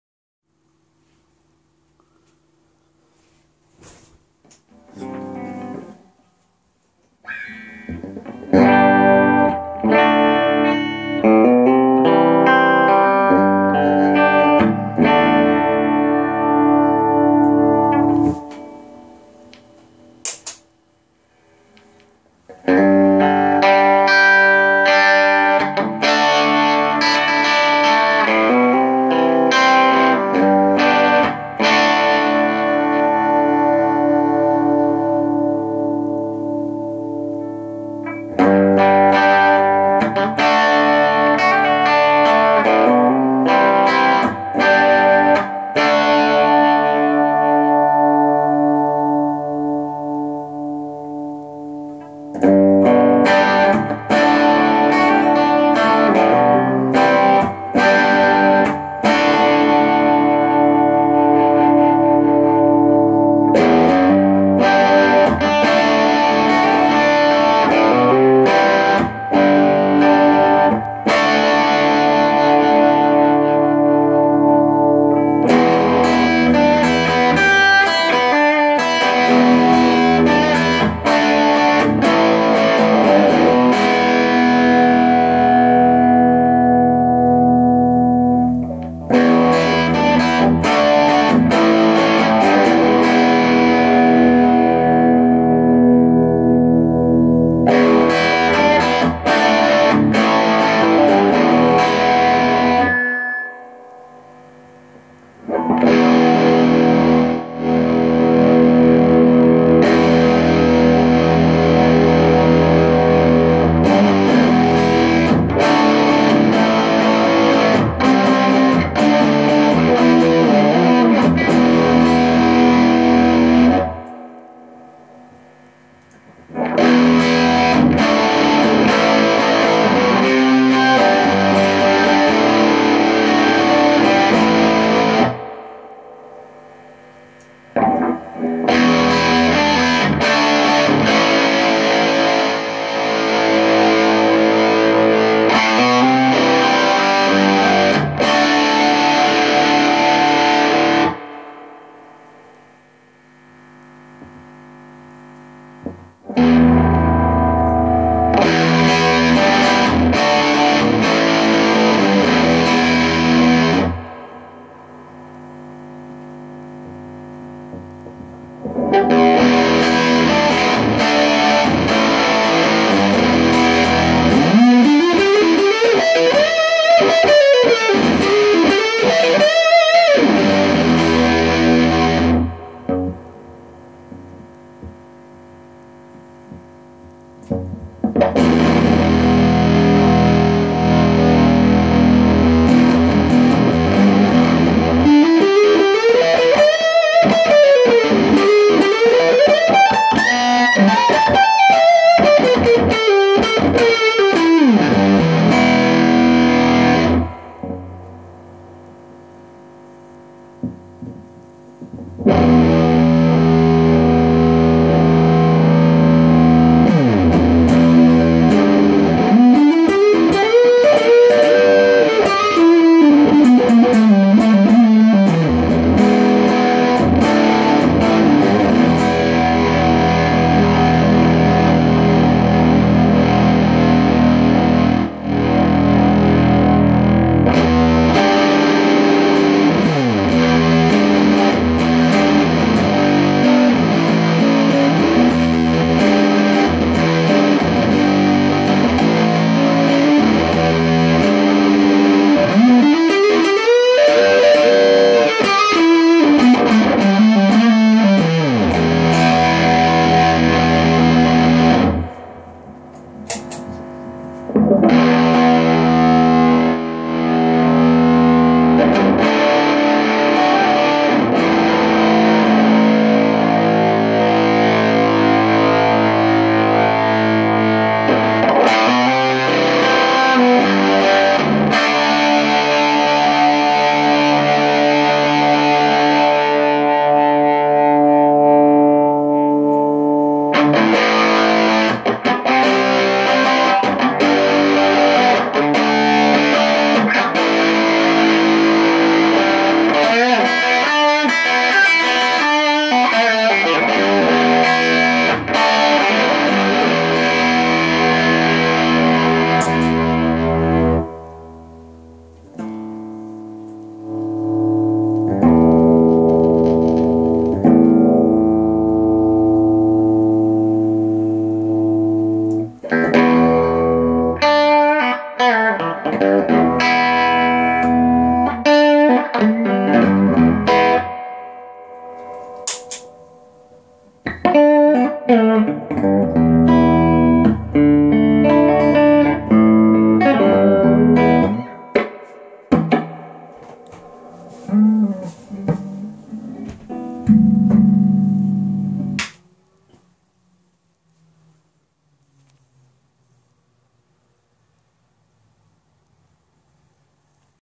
Homemade overdrive pedal
my homemade overdrive pedal
amp: ENGL Screamer 50 (all tube)
guitar: 1996 Epiphone Les Paul custom black beuty 3